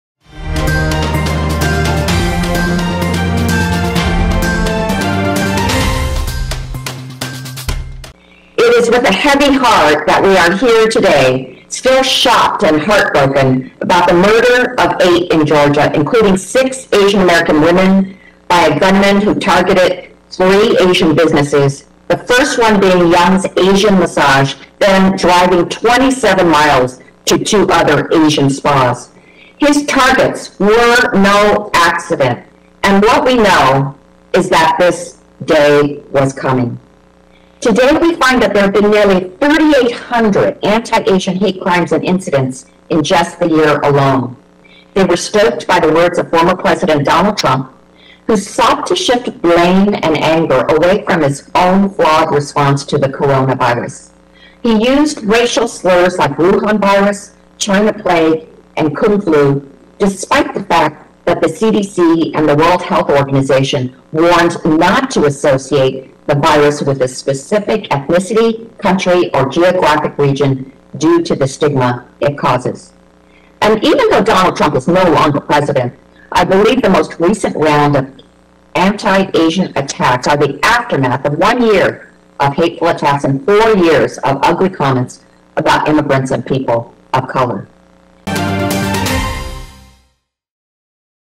Rep Judy Chu Blames Trump for Anti Asian Violence
Rep. Judy Chu (D-Calif.) on Wednesday blamed former President Trump for contributing to a rise in violence against Asian Americans such the previous day's shootings in Georgia.

"President Trump clearly stoked the flames of xenophobia against AAPIs with his rhetoric,” Chu said during a House Democratic Caucus press conference.

“The CDC and the World Health Organization said that we should all use the official term, COVID-19, in order to make sure that this disease is not associated with a particular geographical location or ethnicity due to the stigma it causes. And President Trump refused to acknowledge that," said Chu, who is the chair of the Congressional Asian Pacific American Caucus.